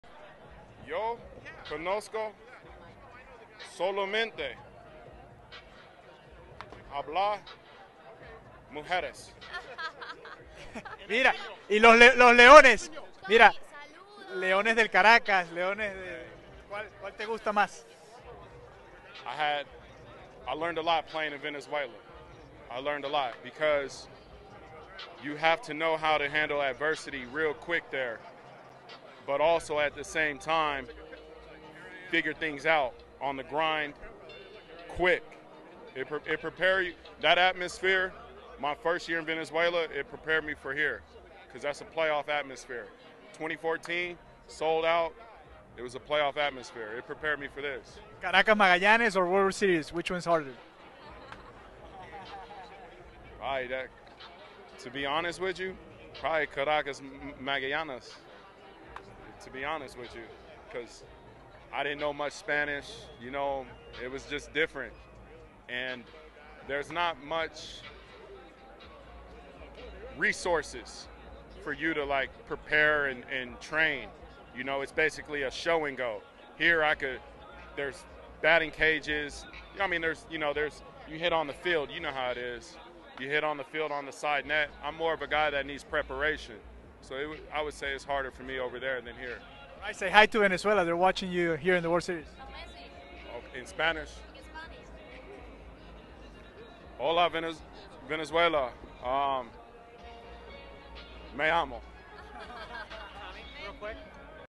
MLB Tommy Pham compara la Serie Mundial con jugar un Caracas-Magallanes 🇻🇪❤️‍🔥
Tommy Pham compara la Serie Mundial con jugar un Caracas-Magallanes.Asegura que la presión de jugar en Venezuela es siempre con “ambiente de playoff”. Pham jugó con los Leones del Caracas en la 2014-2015 y con el Magallanes en la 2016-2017.